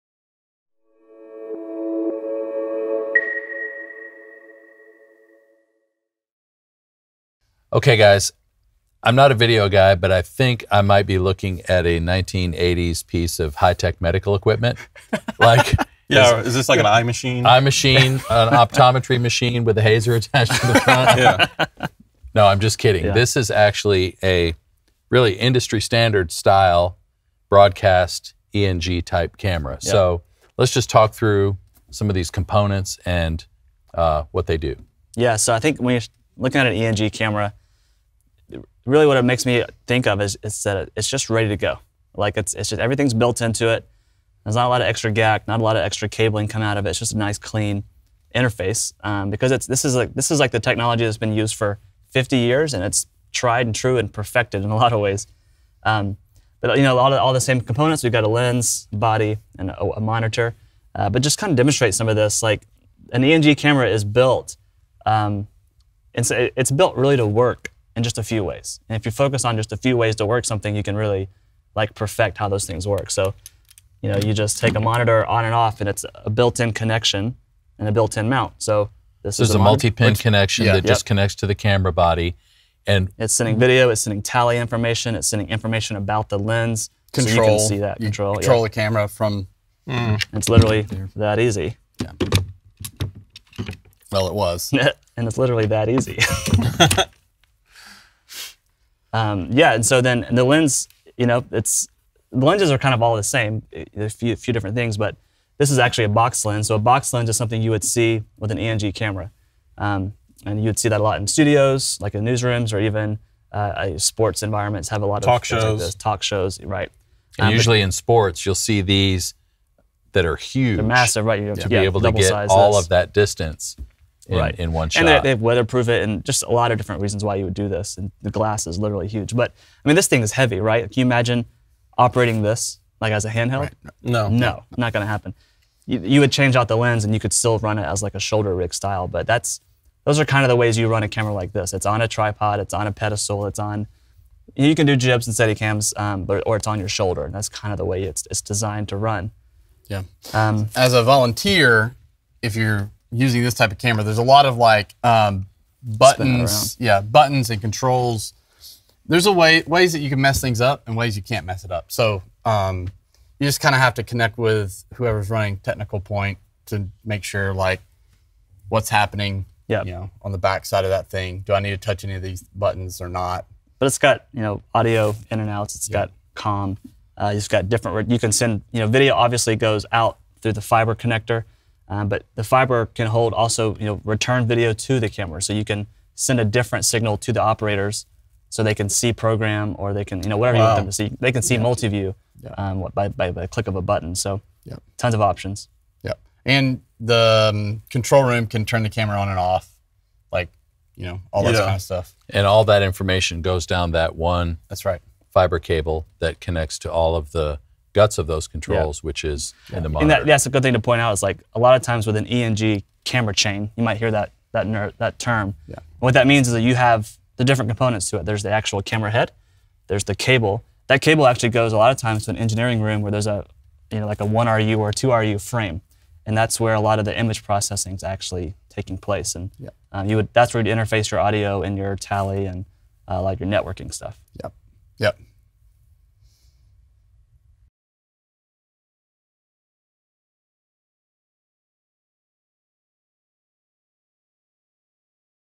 ENG Broadcast Camera Breakdown
Your most common live capture camera. Used around the world in churches, news, sports, and almost all of broadcast. You have to know this camera.

What is MxU?
The all-in-one training platform for church worship and tech teams.